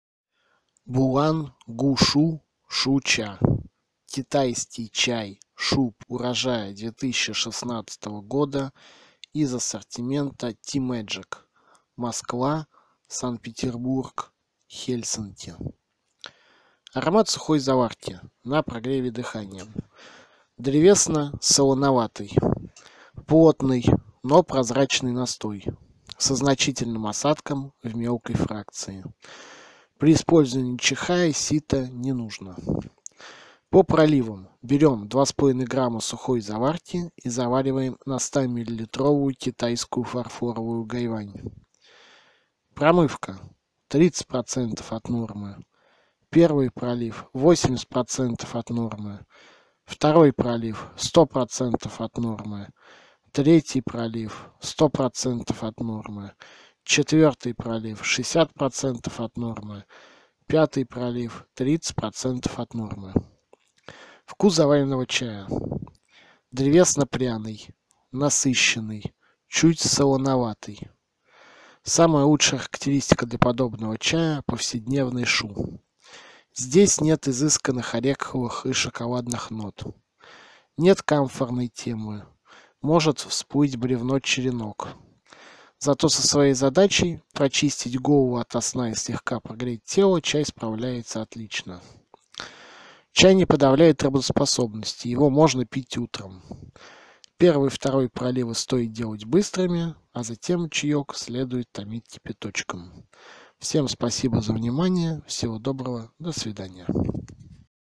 Буланшань гушу шу пуэр. Распаковка и обзор китайского чая пуэр
Bulanshan gushu shu-puer. Unboxing and review of Chinese Puer tea